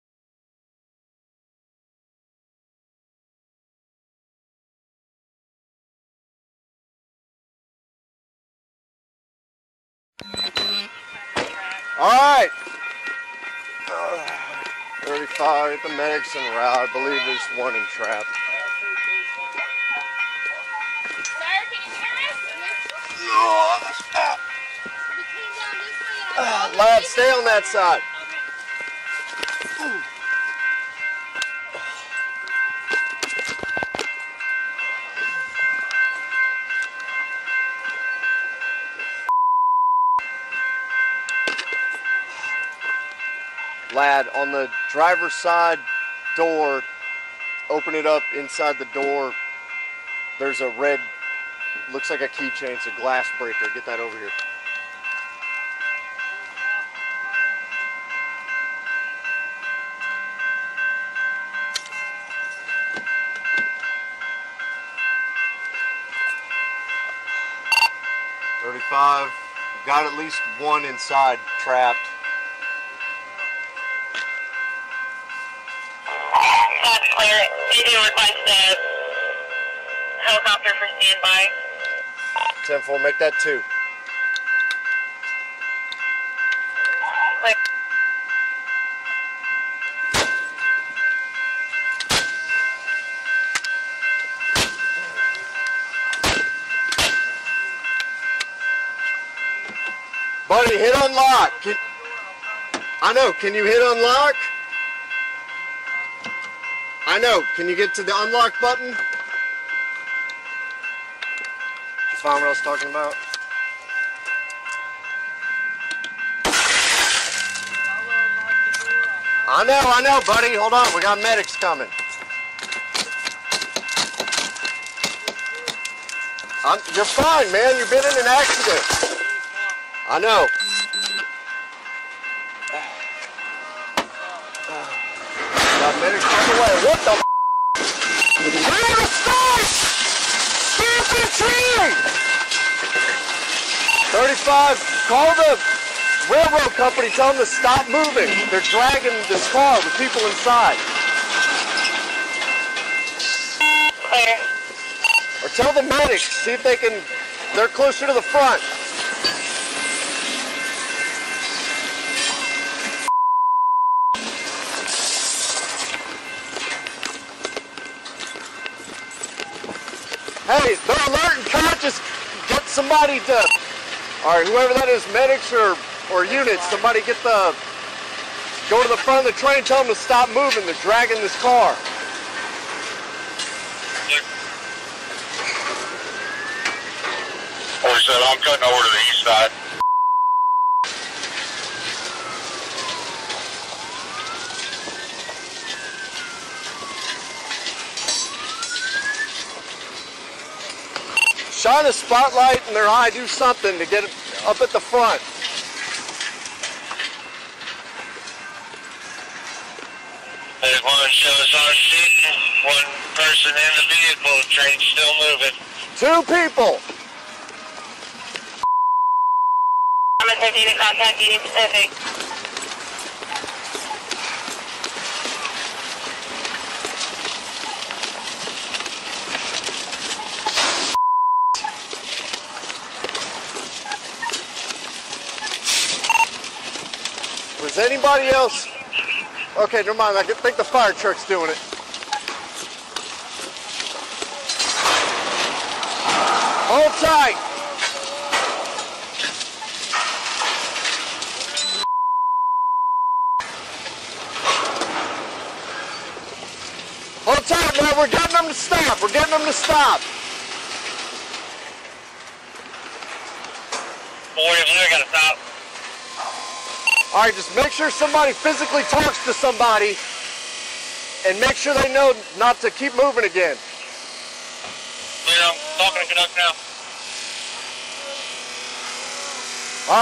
Police Rescue Occupants of Car Being Dragged By Train
Officer Andrew Best and a police trainee from the La Marque Texas Police Department responded to a call of a car vs train and the train started moving after they arrived and began trying to remove the occupants of the car.

The first responders were able to get the train stopped and Both occupants were rescued from the vehicle and airlifted to the hospital